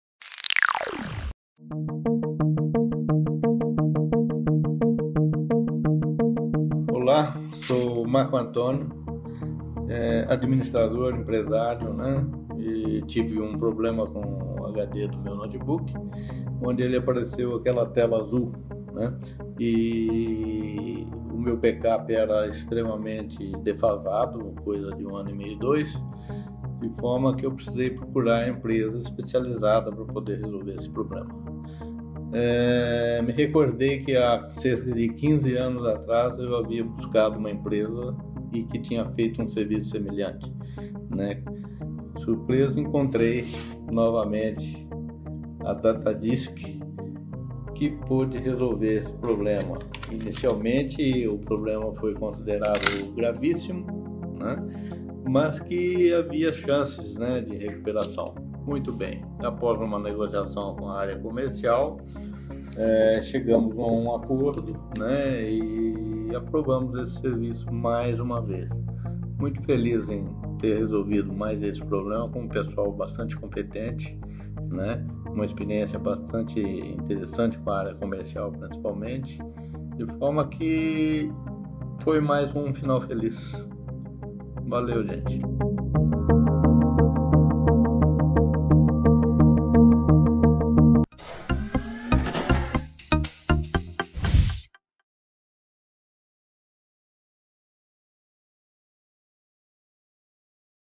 Depois de 15 anos precisei novamente de recuperação de dados e procurei novamente a DATADISK
Recebemos um HD de notebook de 1TB do Sr. Marco Antonio que a 15 anos atrás já tínhamos recuperado dados para ele.
Assista o depoimento e verá que;
Somos a empresa certa para recuperar os seus dados, veja outros depoimentos.

A DATADISK é a única empresa de recuperação de dados no Brasil que possui Laboratório Avançado de Recuperação de Dados.